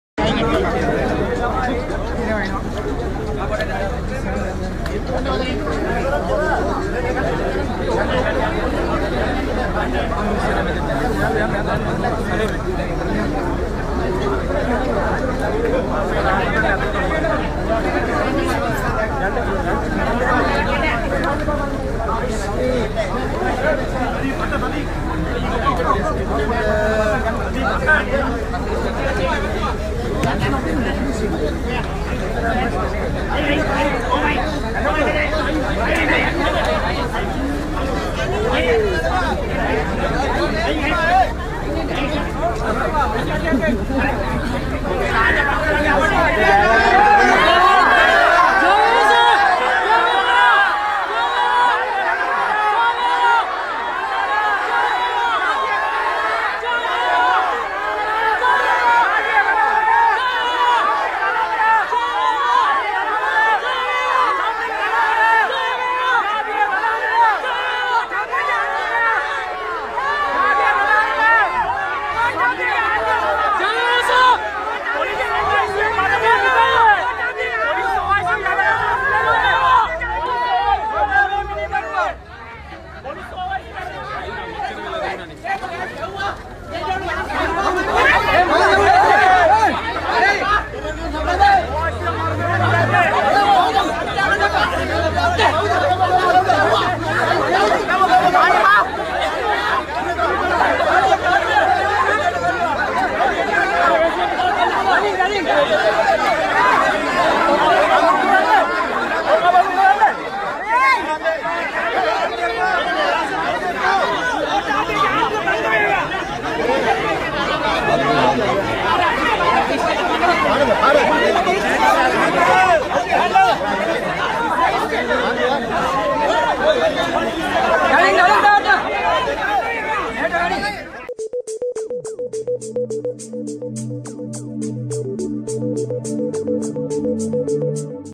හිටපු ඇමැති චම්පිකට ඇප නෑ
Former Minister Patali Champika Ranawaka,who was arrested in connection with the motor accident in 2016 in which a youth was critically injured, was today further remanded till December 24 by the Colombo Magistrate’s Court.